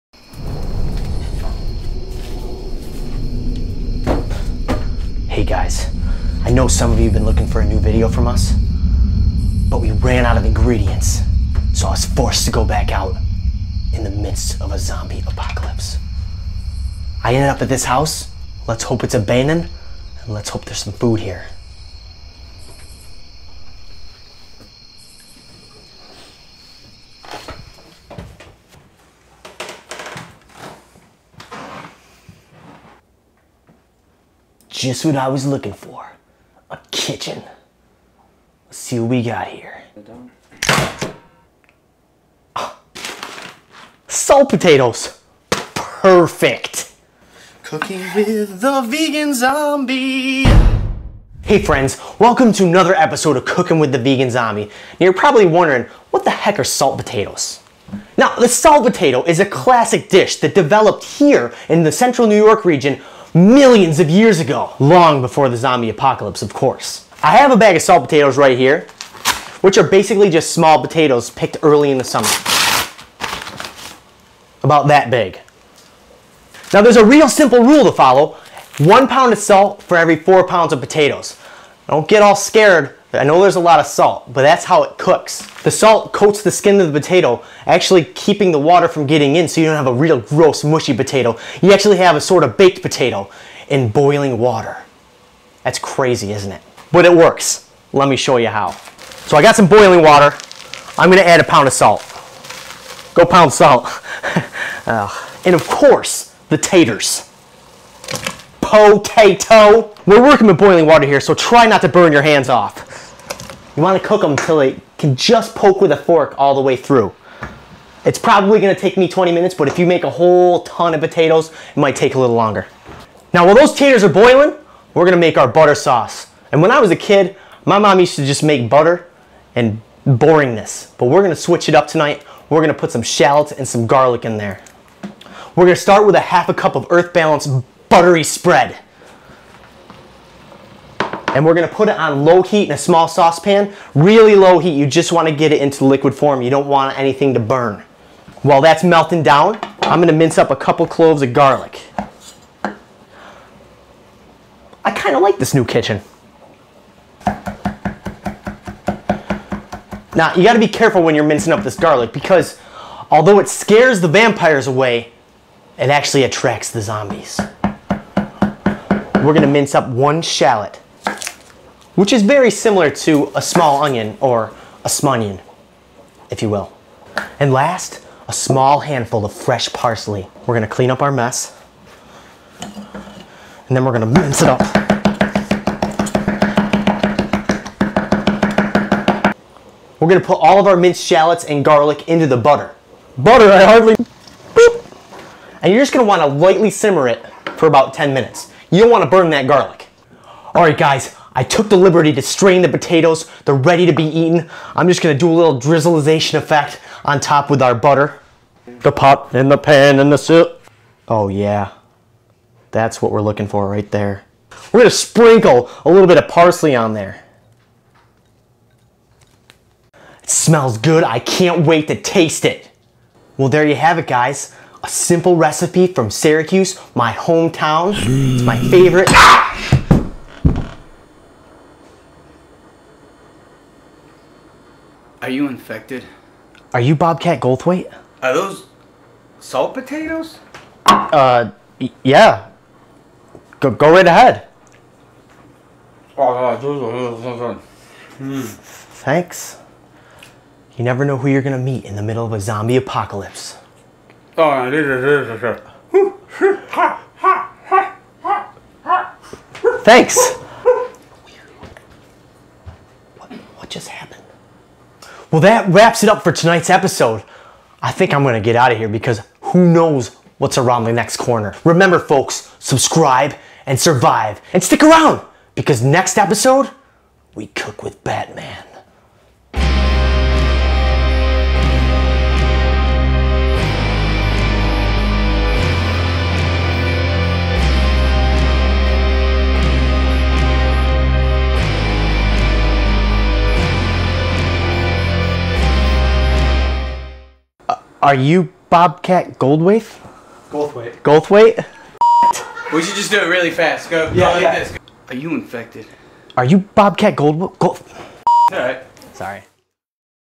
Salt Potatoes Featuring BOBCAT GOLDTHWAIT - ZOMBIE#15
In this video Jon leaves the safe house in search of more ingredients. He finds some salt potatoes. Salt Potatoes originated in Syracuse NY. He also meets up with special guest celebrity, Bobcat Goldthwait.